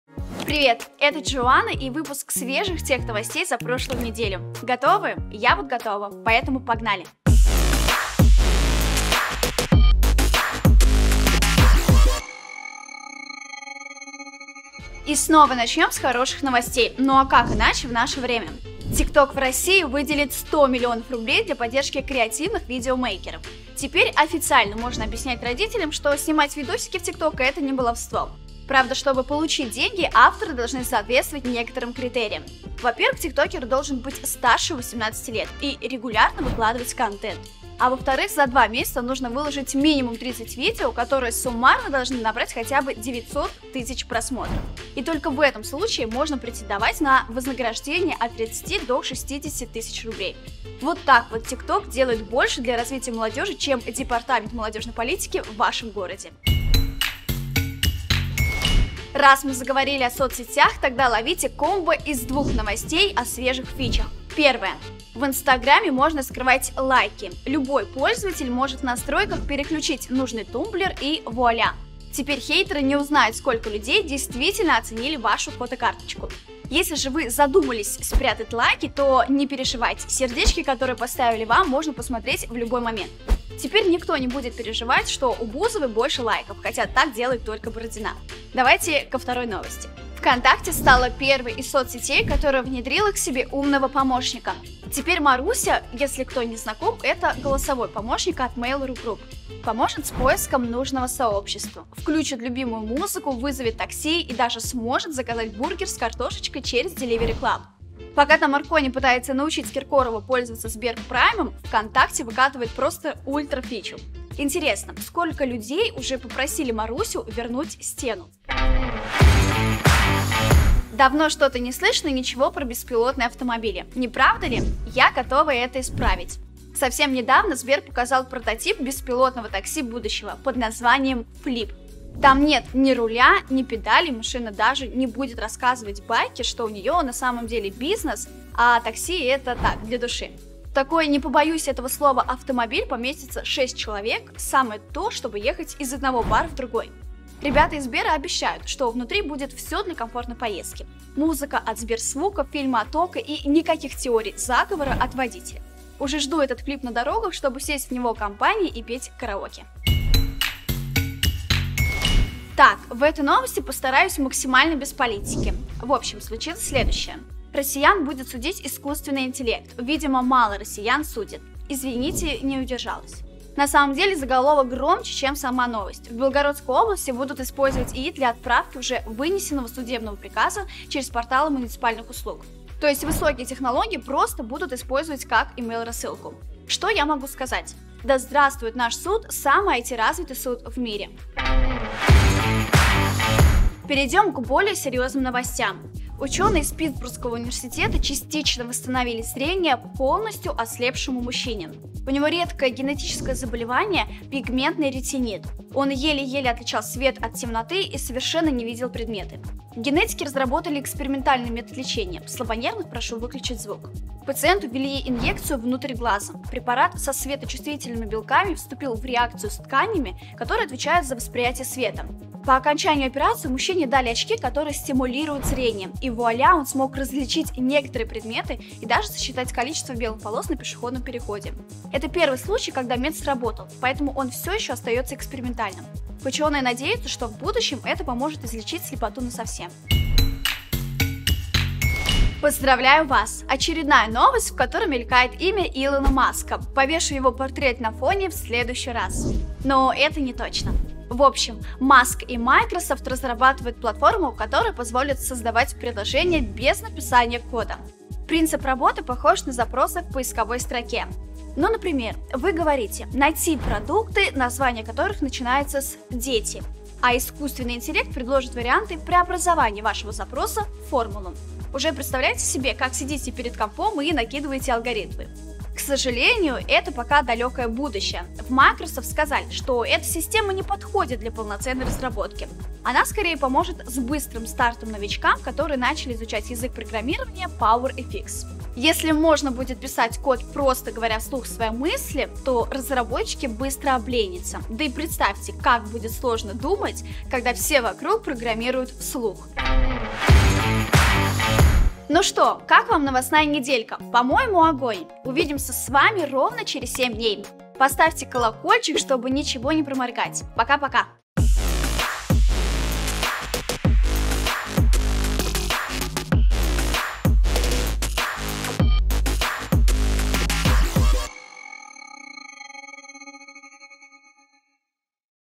WEEKLY ДАЙДЖЕСТ: Беспилотное такси от Сбера, TikTok даст 100 млн самым талантливым // Geekbrains
Обзор последних новостей: TikTok даст 100 миллионов креаторам, Instagram убрал лайки, VK добавил себе Марусю, Сбер показал беспилотное такси, ИИ будет судить россиян, полностью ослепший прозрел, Microsoft и Илон Маск предлагают разрабатывать без написания кода.

00:00 Начало
00:14 TikTok поддержит талантливых
00:58 Instagram скрыл лайки
01:30 VK + Голосовой помощник
02:00 Беспилотный автомобиль Сбера
02:42 ИИ будет судить Россиян
03:13 Новые технологии восстановления зрения
04:02 Коллаборация Microsoft и Илона Маска
05:01 Время, чтобы поставить лайки